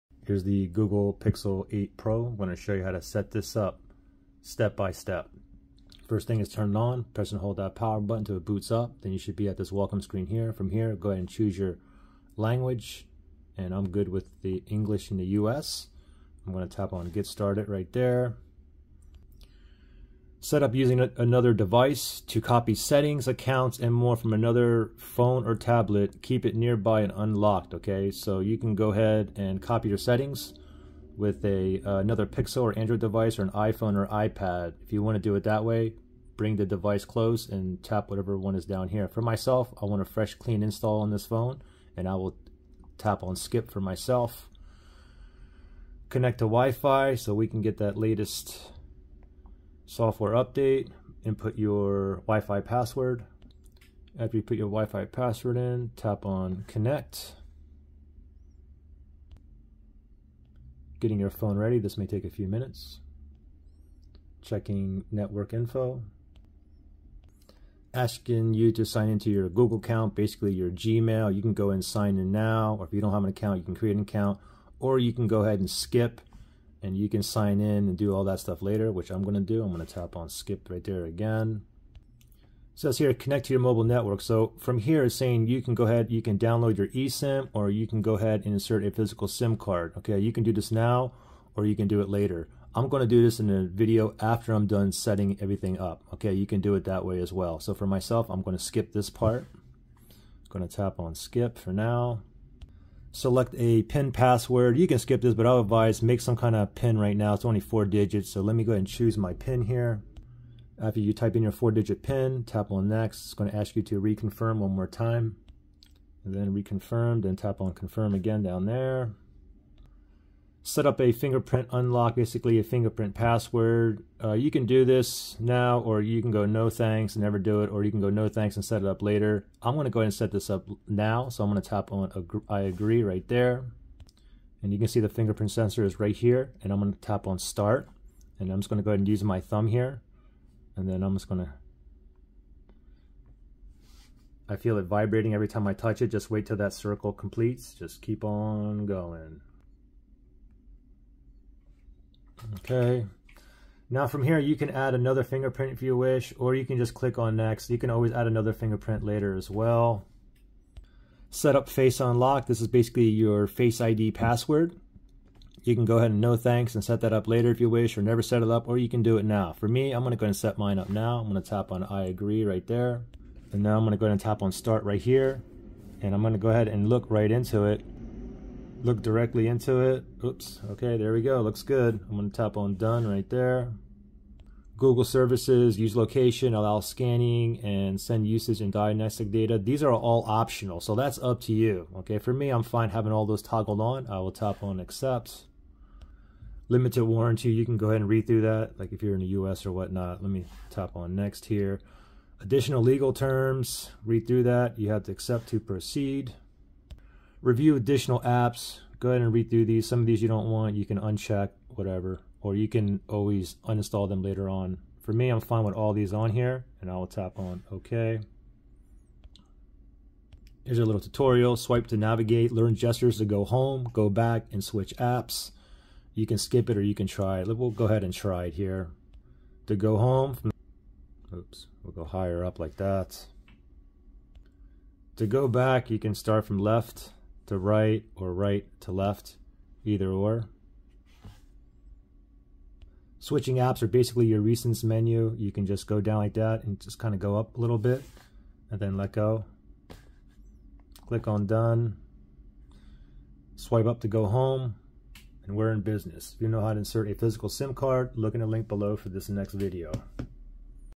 How to Setup Google Pixel 8 Pro (step by step)
I show you how to do the initial setup on the Google Pixel 8 Pro smartphone step by step.

Google Pixel 8 Pro - Unlocked Android Smartphone with Telephoto Lens and Super Actua Display - 24-Hour Battery